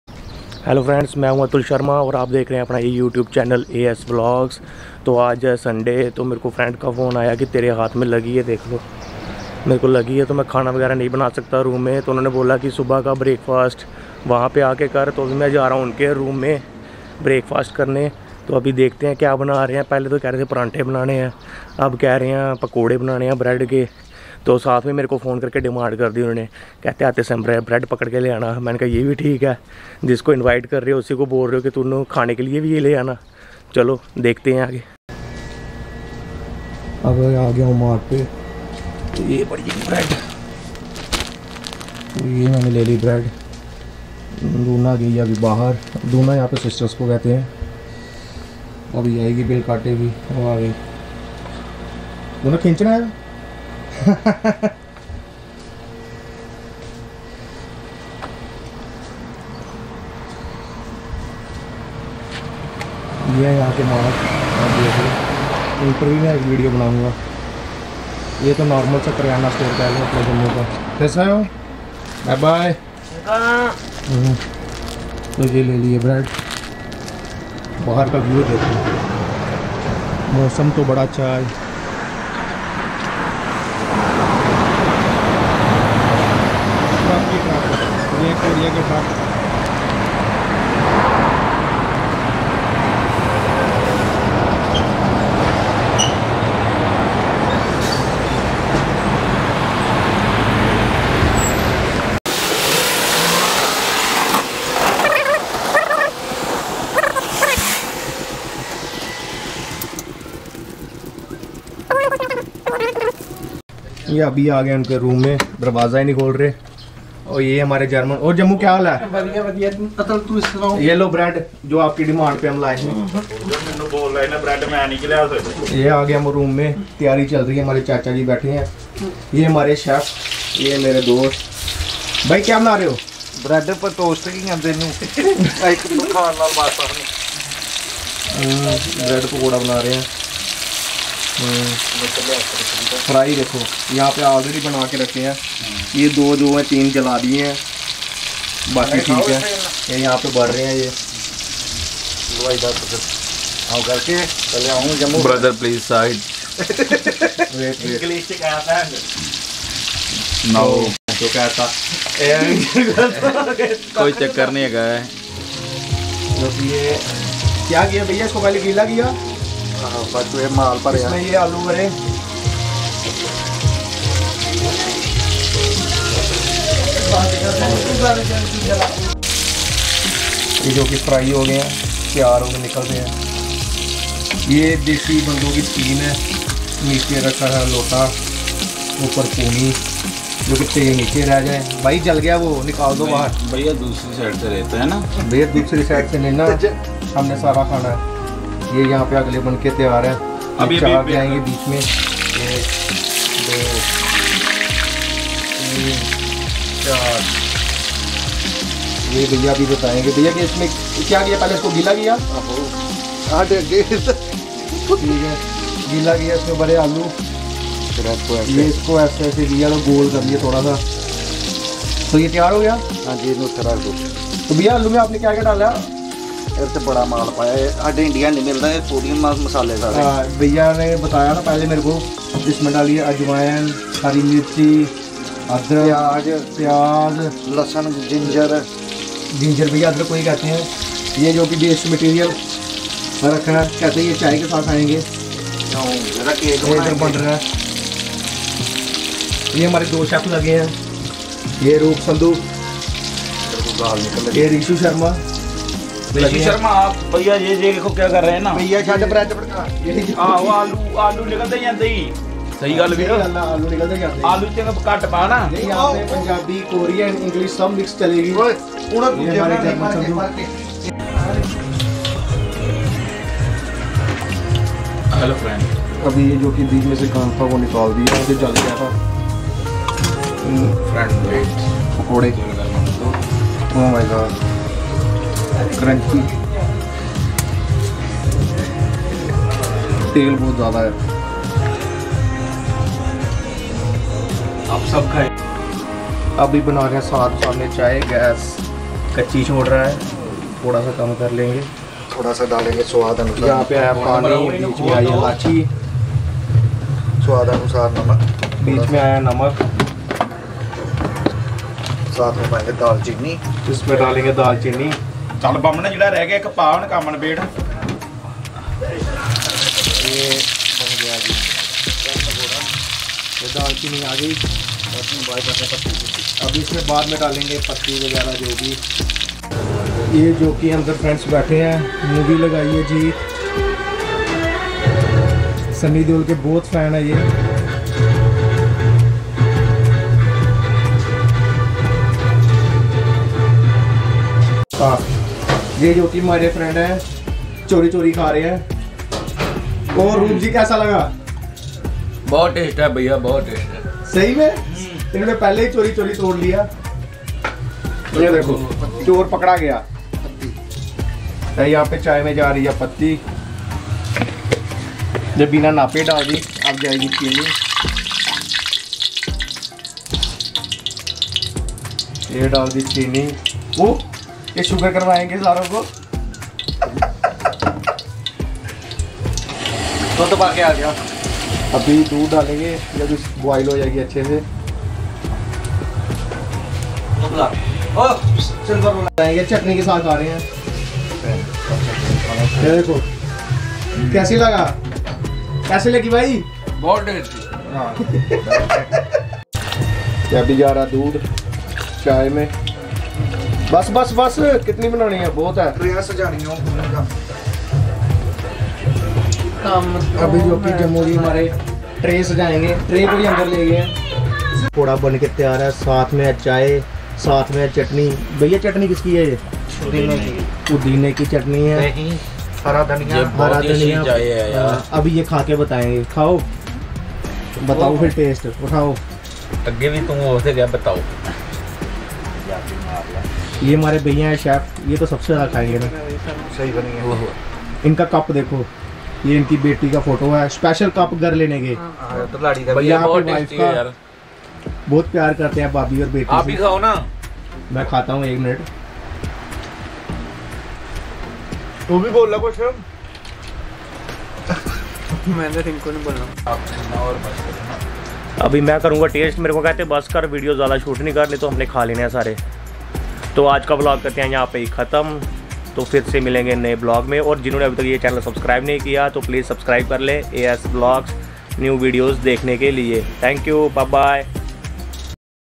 Desi Style Bread Pakoda with tea 🍵//Morning Breakfast // Indians in South Korea 🇰🇷// AS VLOGS
Hi,

My Gears:
My Camera
My Lens
My Tripod